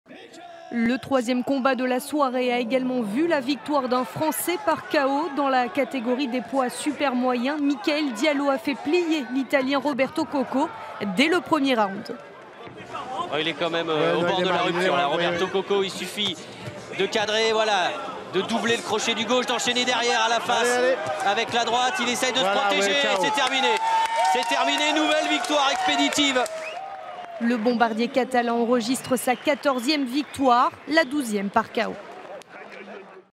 Diallo Mickael expéditif en direct sur CANAL+
" Le Bombardier Catalan "
30/07/2016 Le Cannet en direct sur CANAL +
TKO 1er Round